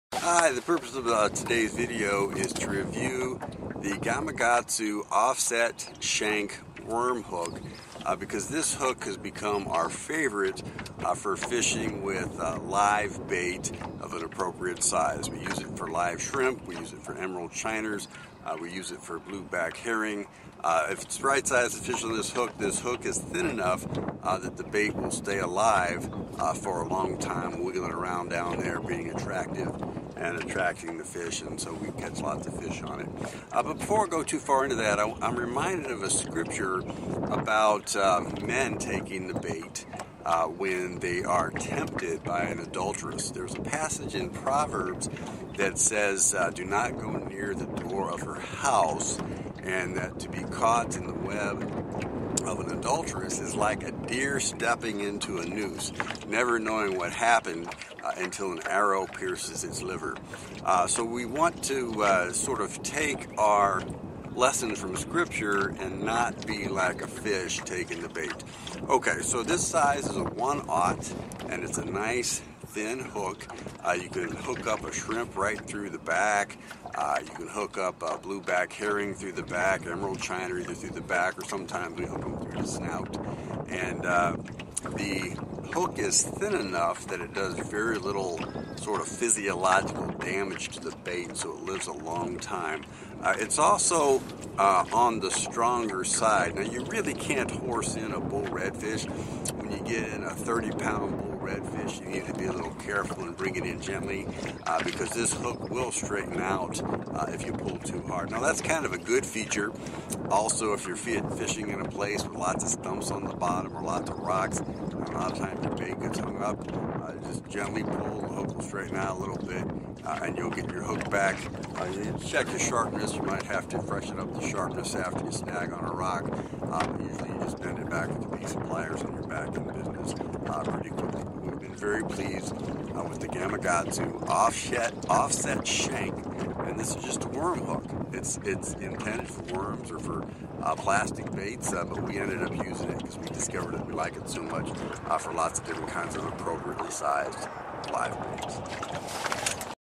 Review of Gamakatsu Offset Shank Worm Hook for Live Bait
Very sharp and thin so bait lives a long time.  Can be iffy on fish over 10 lbs, especially catfish with their large bite force.